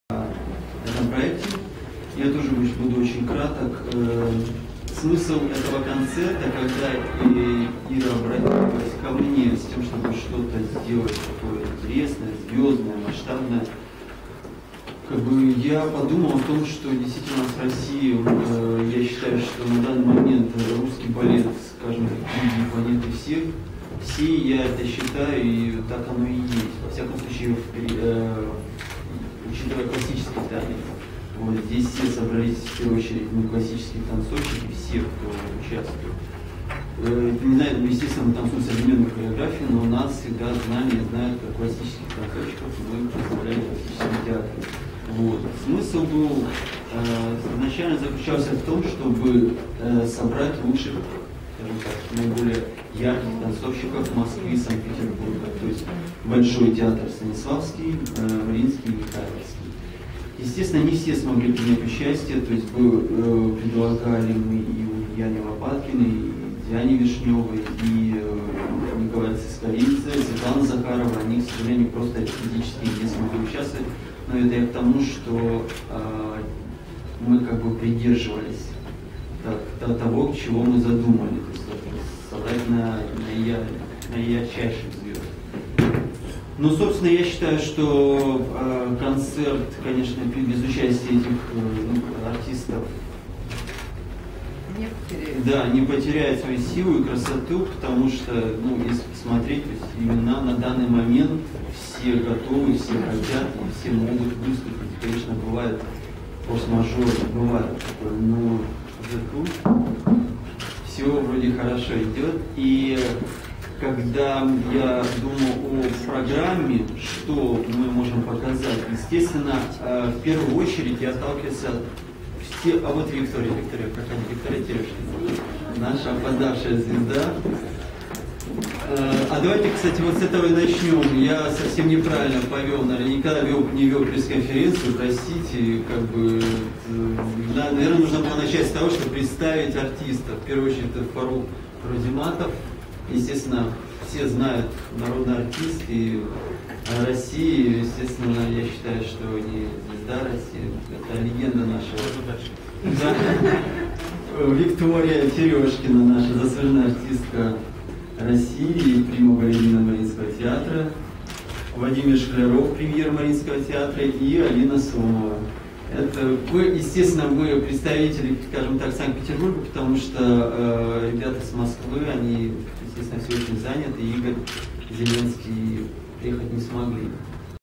Звезды двух столиц. Денис Матвиенко
17 февраля в БКЗ "Октябрьский" в 19 00 состоится уникальный проект, где встретятся на одной сцене лучшие мастера! Это мировые звёзды балета и ведущие солисты Мариинского, Михайловского театров, Большого и театра им. Станиславского и Немировича-Данченко, лауреаты самых престижных российских и международных конкурсов, лауреаты театральных премий Москвы и Петербурга "Золотая Маска" и "Золотой Софит", лауреаты специальных балетных международных премий...

- народные артисты России Игорь Зеленский и Фарух Рузиматов, заслуженные артисты России Игорь Колб, Виктория Терешкина, Ирина Перрен, артисты Наталья Осипова, Иван Васильев, Анастасия Матвиенко, Денис Матвиенко, Екатерина Крысанова, Вячеслав Лопатин, Алина Сомова, Владимир Шкляров, Марат Шемиунов, Наталья Сомова, Cемен Величко, Kристина Шапран...
История творческой жизни каждого из этих танцовщиков является зримой страницей в летописи русского балета. Каждое из этих имен - это отдельное направление, удивительный синтез театрального и балетного искусства, а номера в их исполнении - это отдельный спектакль.